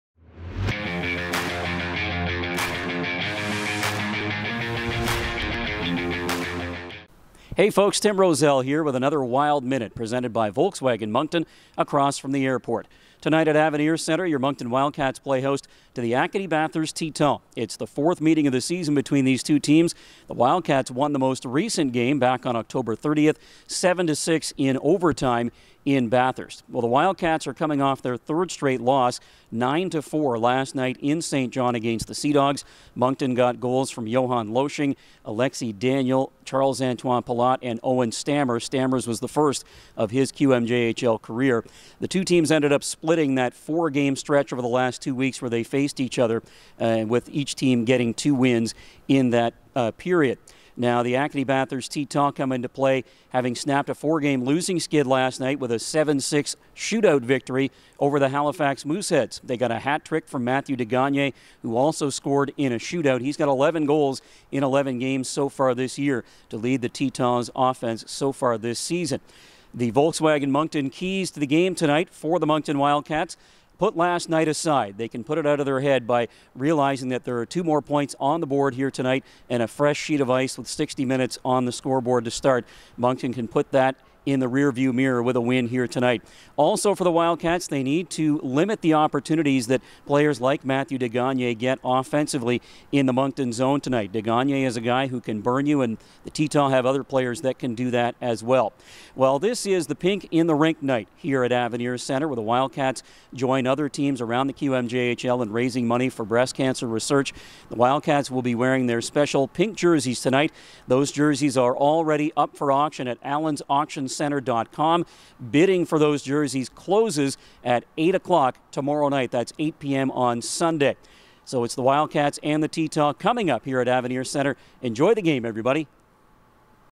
Wild Minute | November 14, 2020
It's Pink in the Rink night at the Centre Avenir Centre!

Team broadcaster Tim Roszell has all the details you need to know heading into tonight's contest against the Acadie-Bathurst Titan